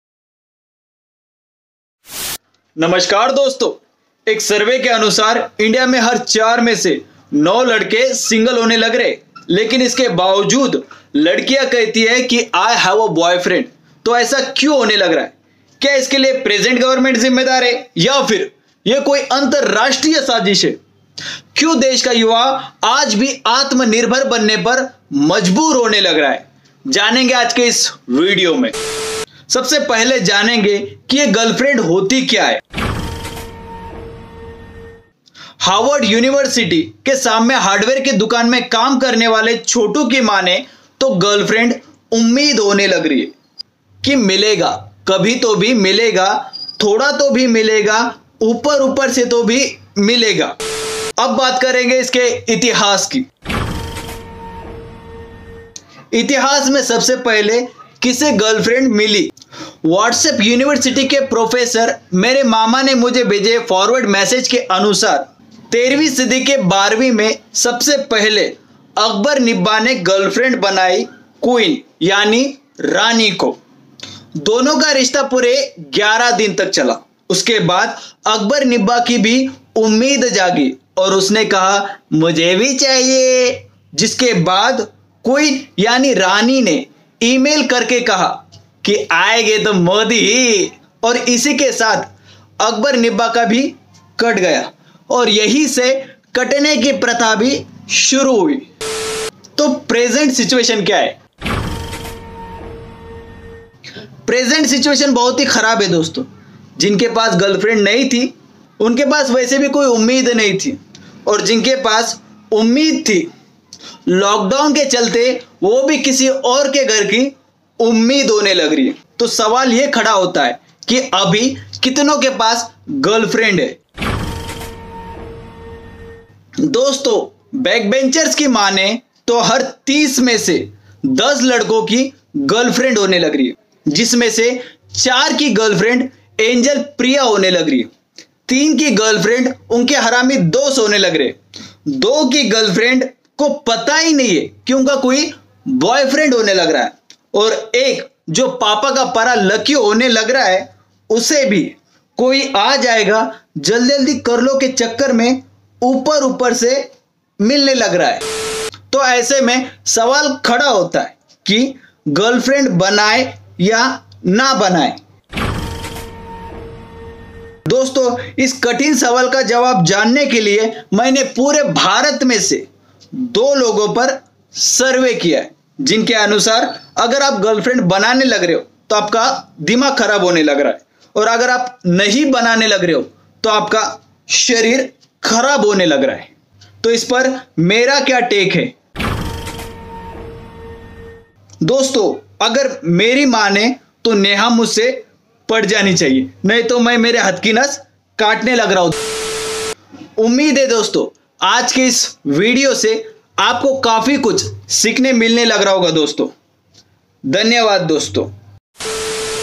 Why Indian Guys are Single | Analysis By Dhruve Tripathi
Why Most of the Guys are Single when every second girl is committed

ये एक experimental video है, उम्मीद है आपको पसंद आएगी..😁😉
i really love Dhruve Rathee videos , बहोत कुछ सीखने मिलता है ।❣️
ये बस parody video है तो offence मत हो जाना 🙏

मुझे ढूंढ रहे हो क्या ? यहां मिल जाऊंगा.

tags